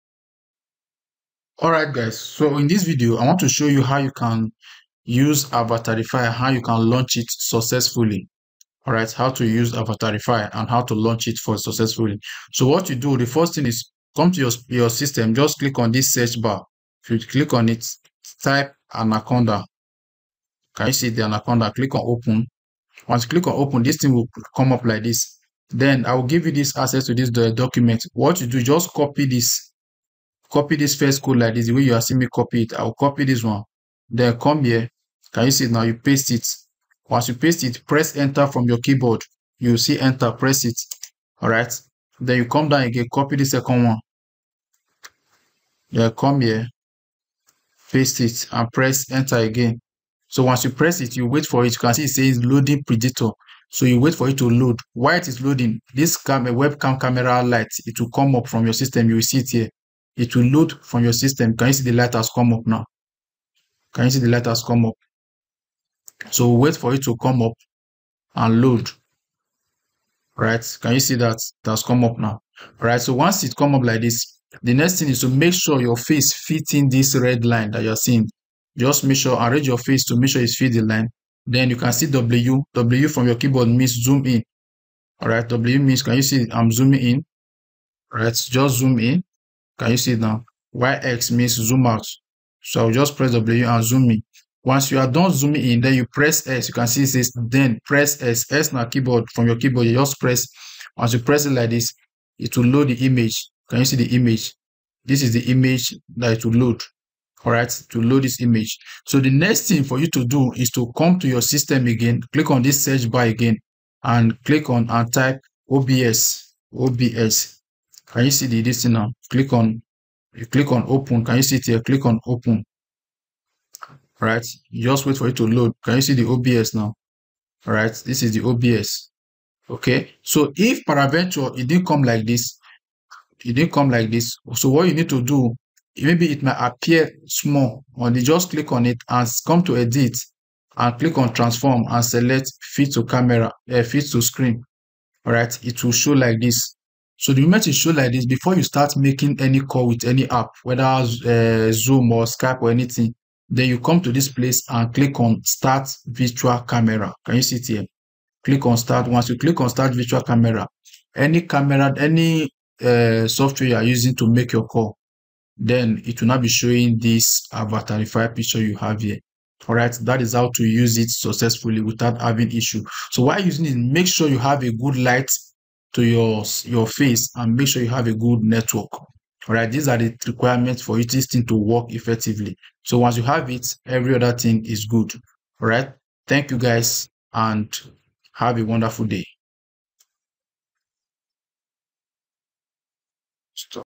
How to use Avatarify Deep Video Call Software
🚀 Ready to take your video calls to the next level? Say hello to Avatarify Deep Video Call Software! 🌟 In this comprehensive tutorial, we'll walk you through the ins and outs of using Avatarify. This video will guide you on how to use Avatarify

🎓 Whether you're a professional looking to add a touch of fun to your meetings or just want to spice up your virtual hangouts with friends, Avatarify is the tool for you. Follow our tutorial and elevate your video calls to a whole new level!